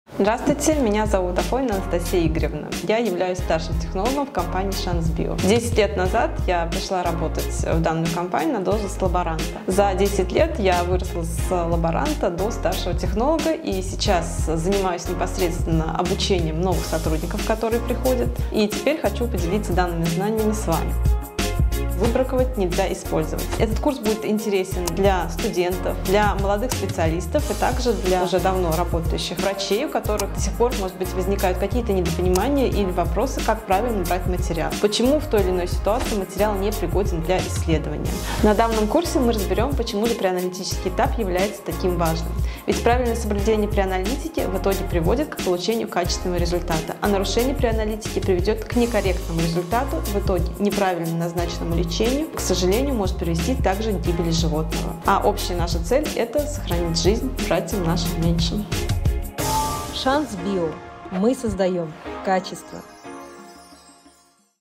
Познакомьтесь с Афониной Анастасией: старшим технологом Шанс Био
Познакомьтесь с Афониной Анастасией: старшим технологом Шанс Био и лектором нашего нового курса. Но о курсе - чуть позже 🤫

10 лет назад Анастасия пришла к нам работать лаборантом и поняла, что здесь - ее семья 🫶

За годы она выросла в должности от лаборанта до старшего технолога. А еще помогает обучать и адаптировать к работе молодых сотрудников. 👩‍🔬

А теперь будет делиться знаниями с вами!
Курс "Выбраковать нельзя использовать" будет полезен для студентов, молодых врачей, а также врачей, у которых есть вопросы, как правильно брать материал.

Почему материал может быть непригоден для исследования? Когда можно получить некорректный результат? Обо всем этом узнаете на курсе!

А 12 августа приходите на бесплатный вебинар с Анастасией. Познакомимся поближе и обсудим такую важную преаналитику в прямом эфире 📲

🎓Лицензия на осуществление образовательной деятельности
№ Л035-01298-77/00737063

Шанс Био: 28 лет в лабораторной диагностике